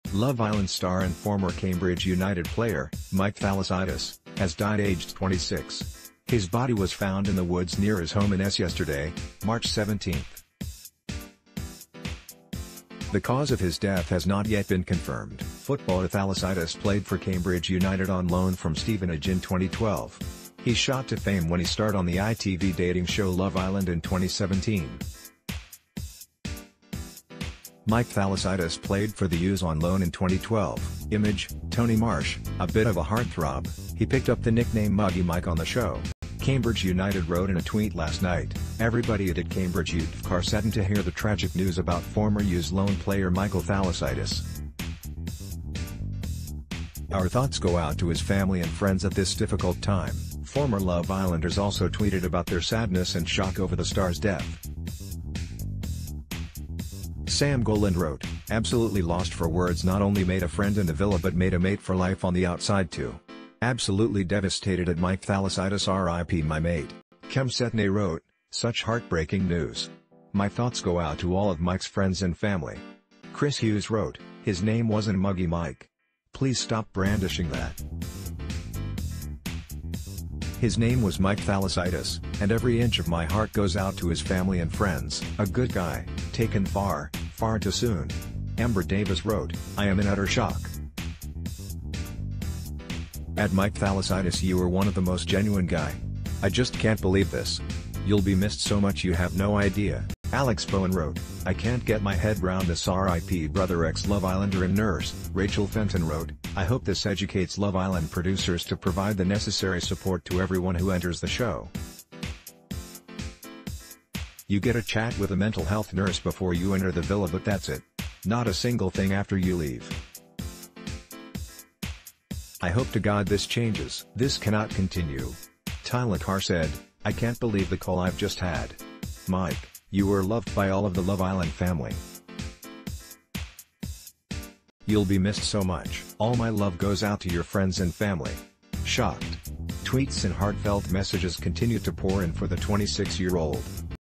Mike Thalassitis: Tributes paid to Love Islander and former U's loan player | Make Money Playing ...
Mike Thalassitis: Tributes paid to Love Islander and former U's loan player | Make Money Playing Video Games!
Love Island star and former Cambridge United player, Mike Thalassitis, has died aged 26. His body wa...